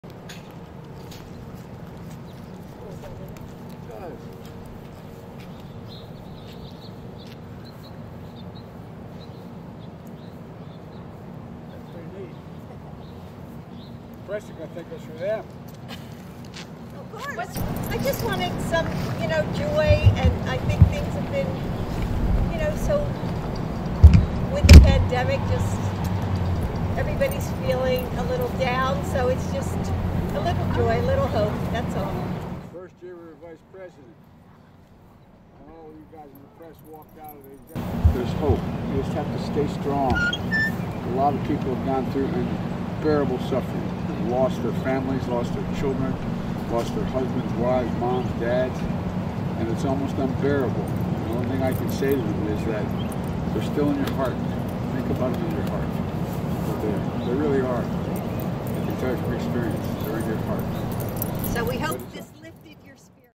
Bidens have message of hope on COVID Valentine's
President Biden and his wife and their two dogs take an early morning stroll on the front lawn of the White House Tuesday to survey their Valentine's message to the world.